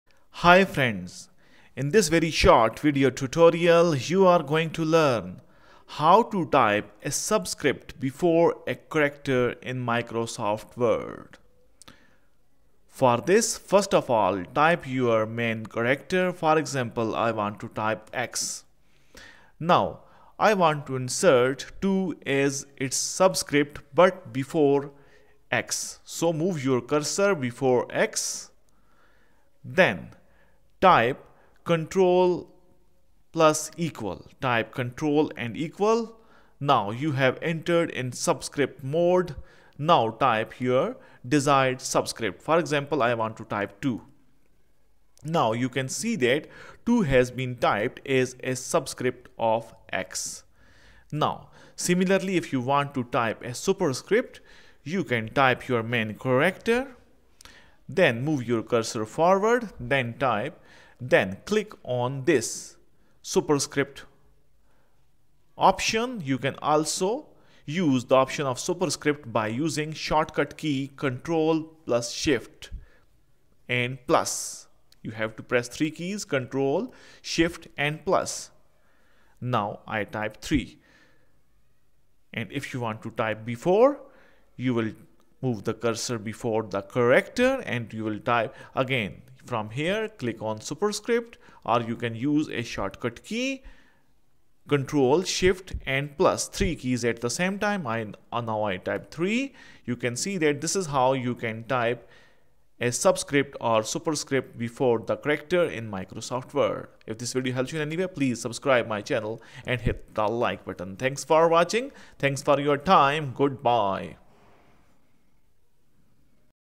How To Type Subscripts and Superscripts in Before the Character MS Word | Subscript Before Letter
This tutorial of Microsoft Word shows how to insert subscript and superscript Before any character in Microsoft word.
This tutorial may also work in Microsoft Word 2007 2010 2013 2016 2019 and 365